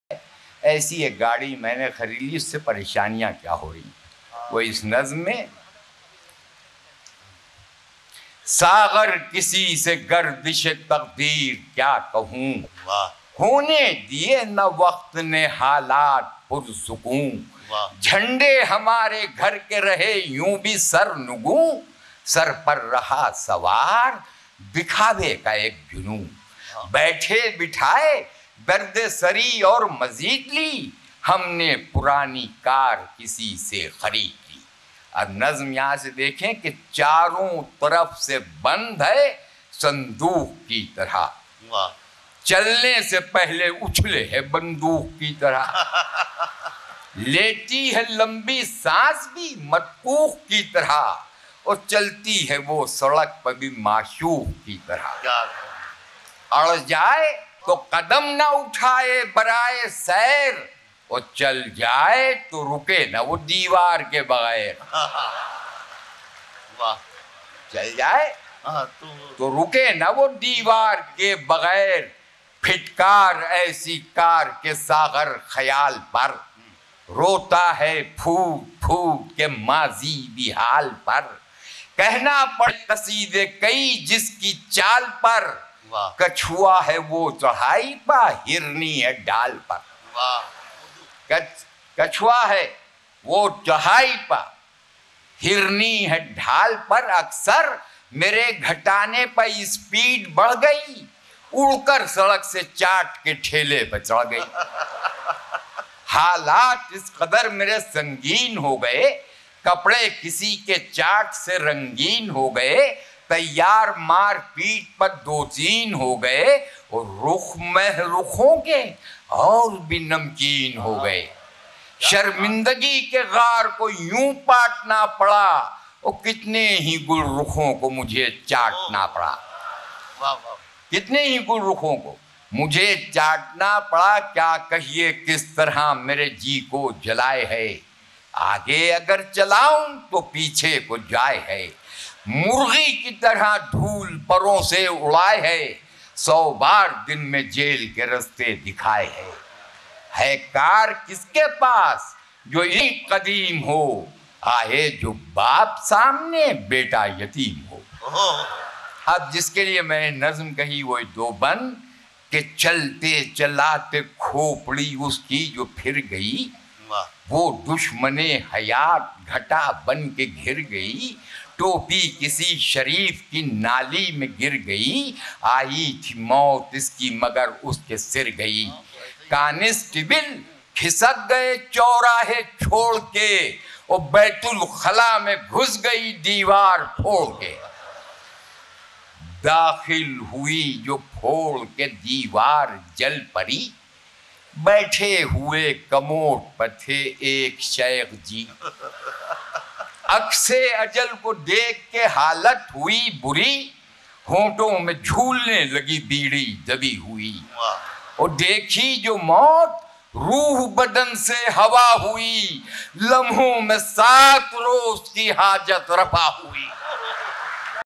Purani Car | پرانی کار | पुरानी कार | Saghar Khayyami
This is Jashn of Urdu at Jashn-e-Bahar '99.